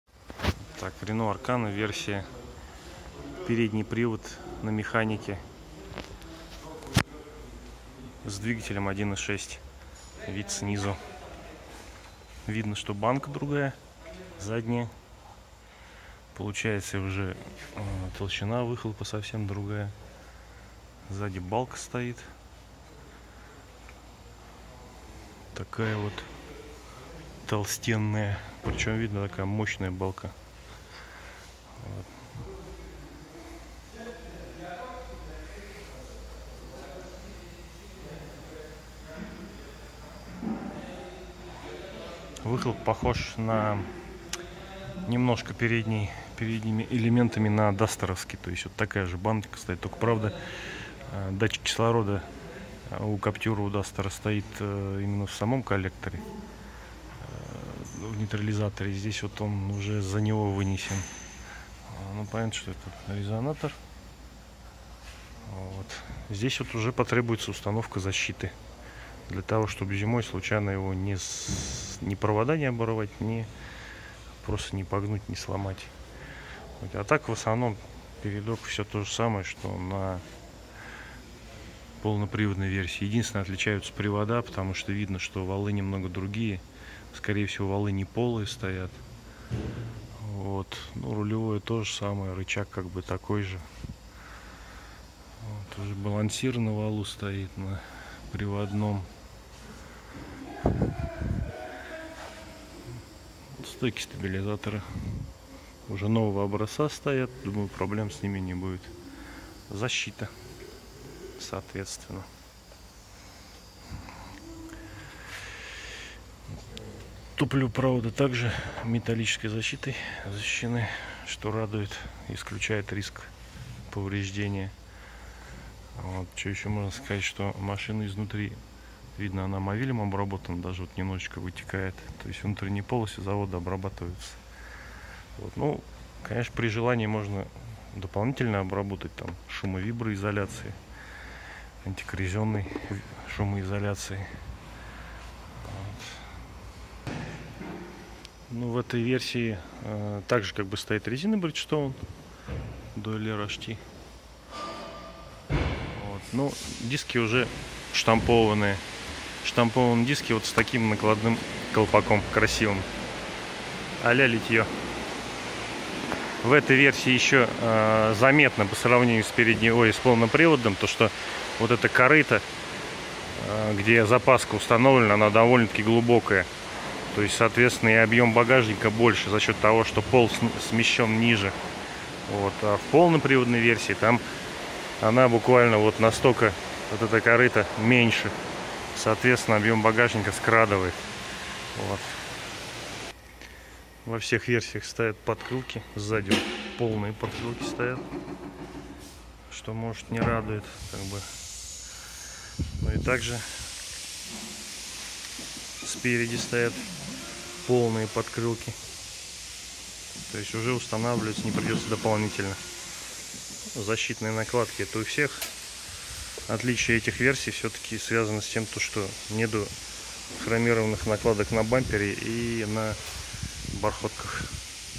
Рено Аркана 1,6 МКПП 4х2 вид снизу
Небольшое сравнение отличий с полноприводными и версиями